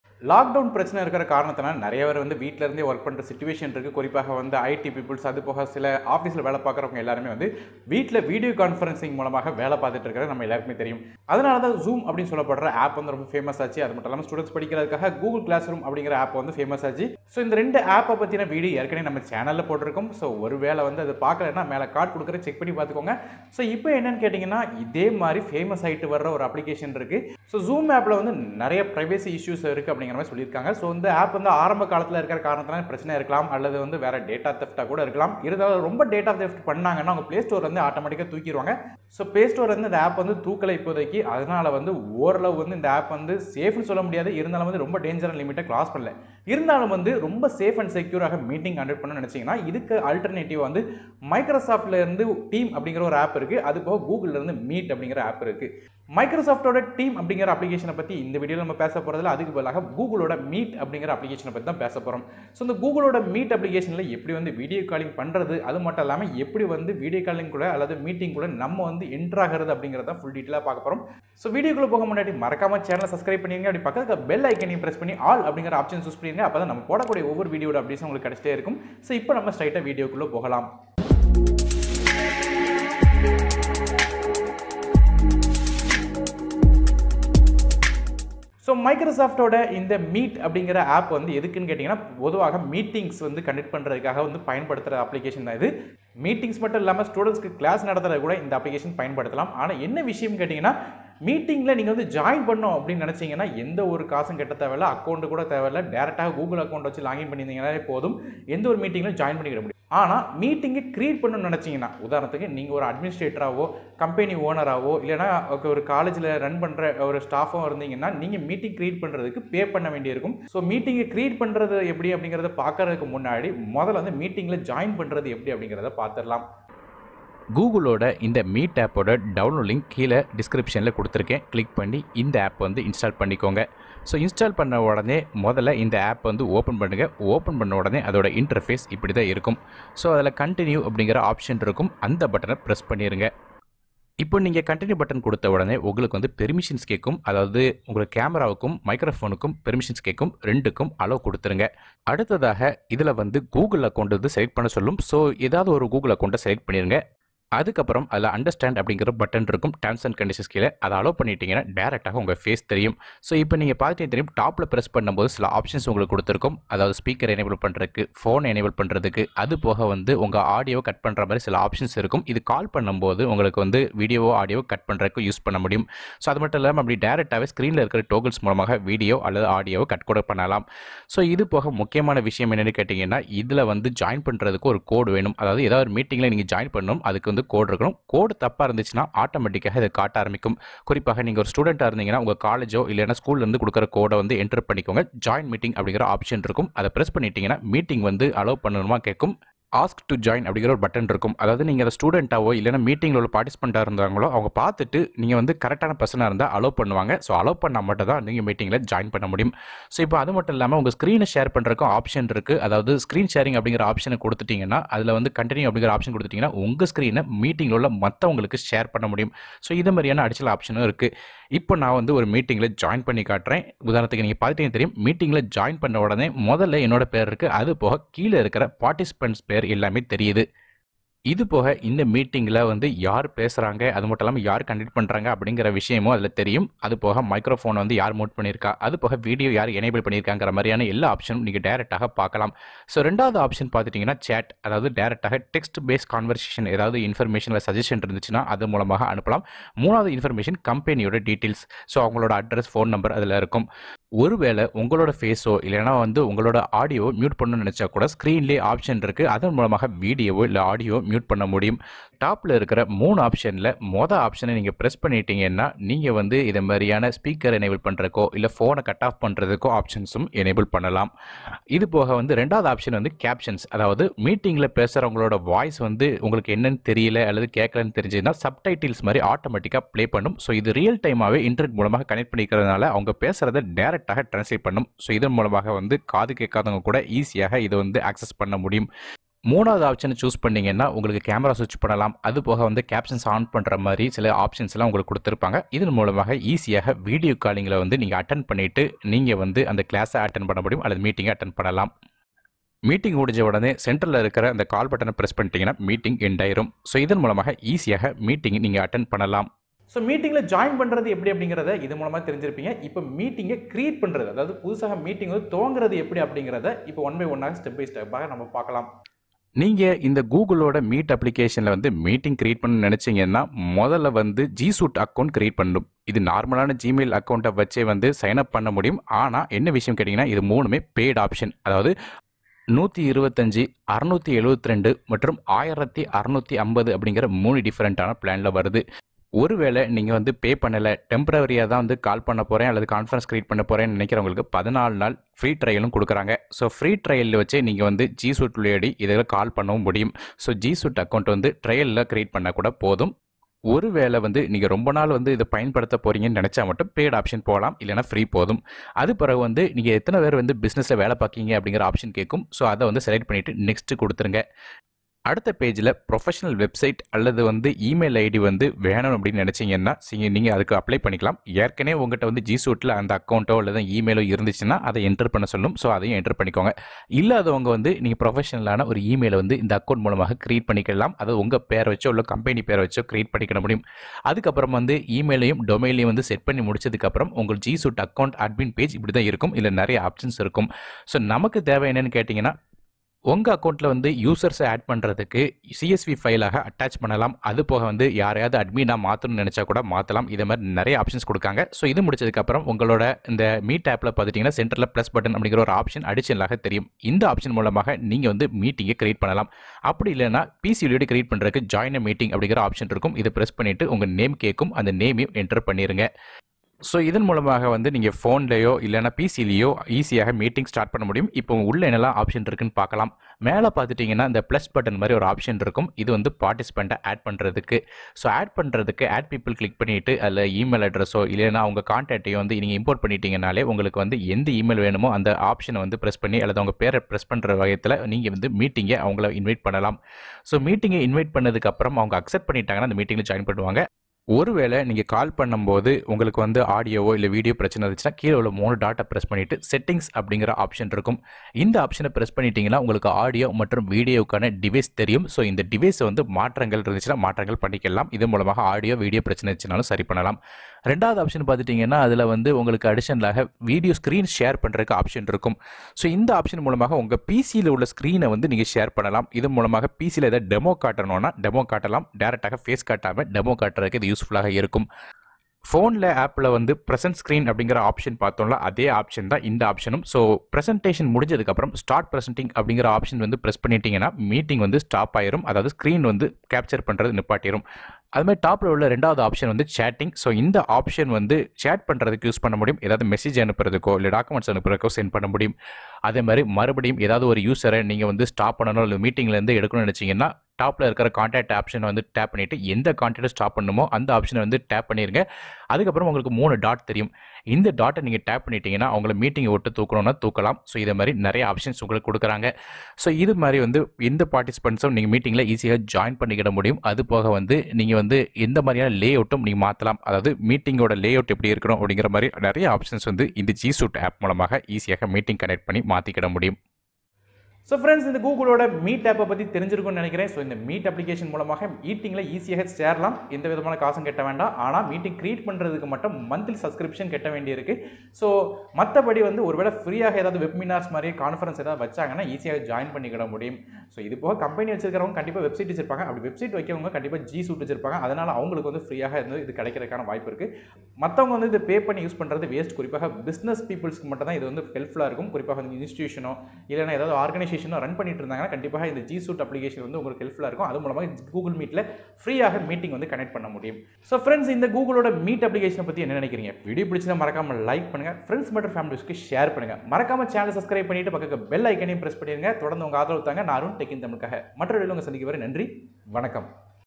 Google Meet App பயன்படுத்துவது எப்படி - How to Use Google Meet
Google Meet App எப்படி பயன்படுத்துவது. மேலும் video conferencing, meeting, online teaching class இதை வைத்து எப்படி எடுப்பது என பார்க்கலாம். How to use Google Meet app in Tamil for teaching, conference, meeting etc.